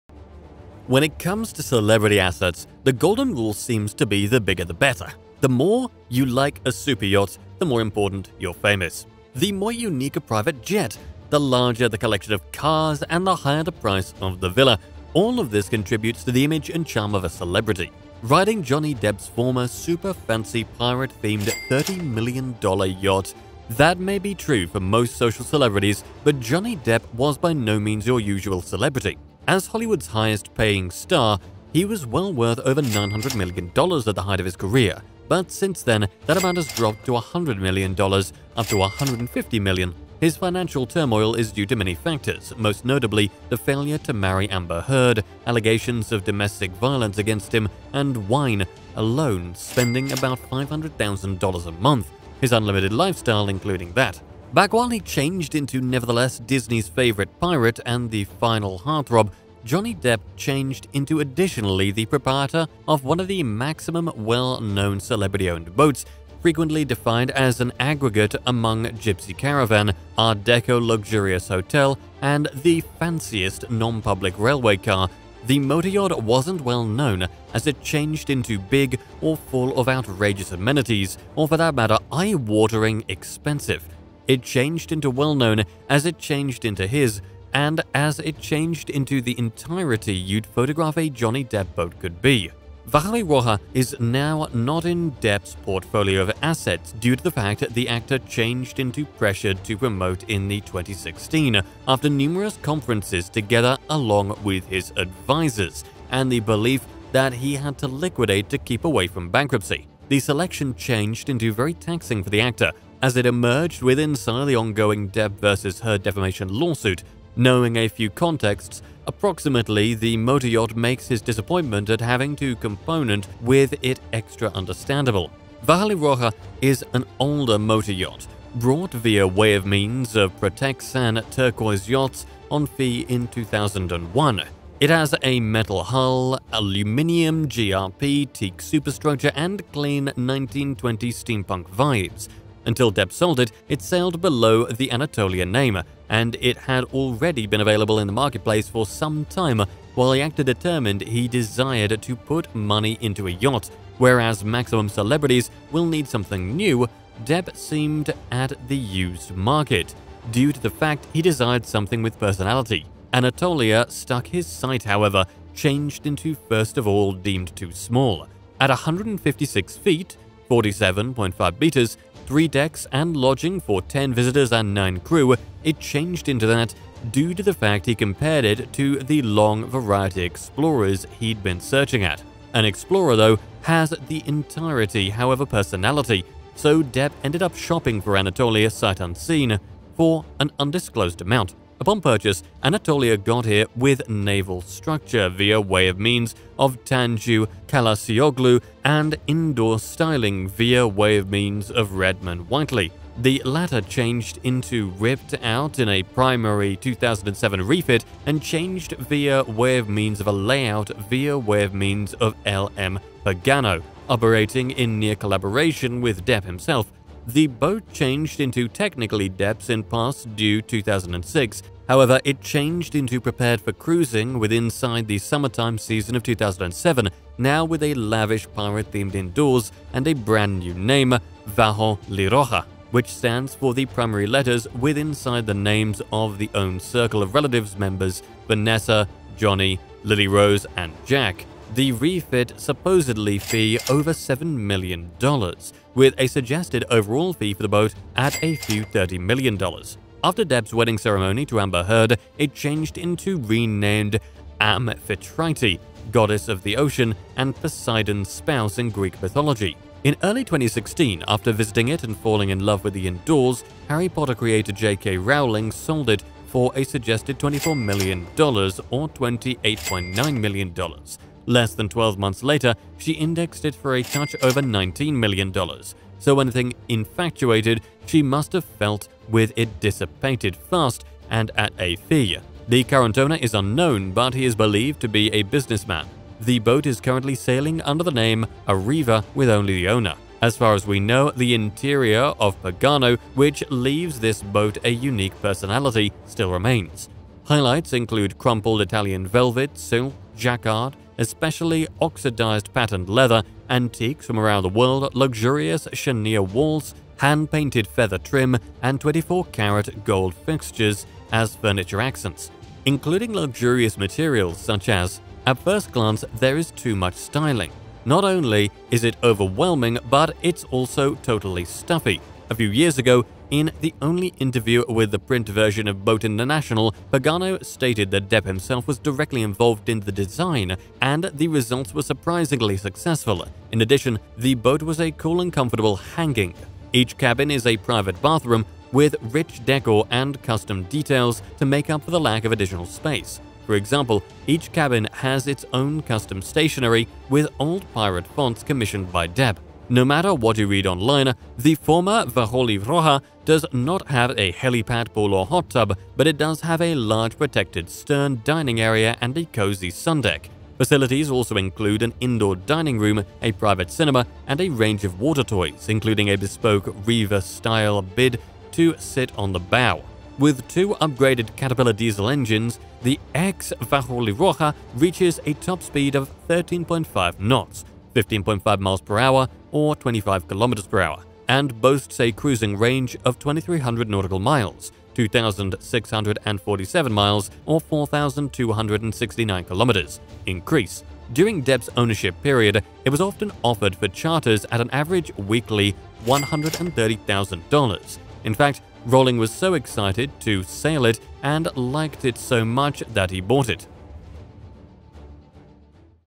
Riding Johnny Depp's Former Super Fancy Pirate-themed $ 30 Million Yacht Vajoliroja
As Hollywood's highest-paying star,  he was well worth over $ 900 million at the height of his career, but since then that amount has dropped to $ 100 million, up to $ 150 million. .. His financial turmoil is due to many factors, most notably the failure to marry Amber Heard, allegations of domestic violence against him, and wine alone spending about $ 500,000 a month. His unlimited lifestyle, including that.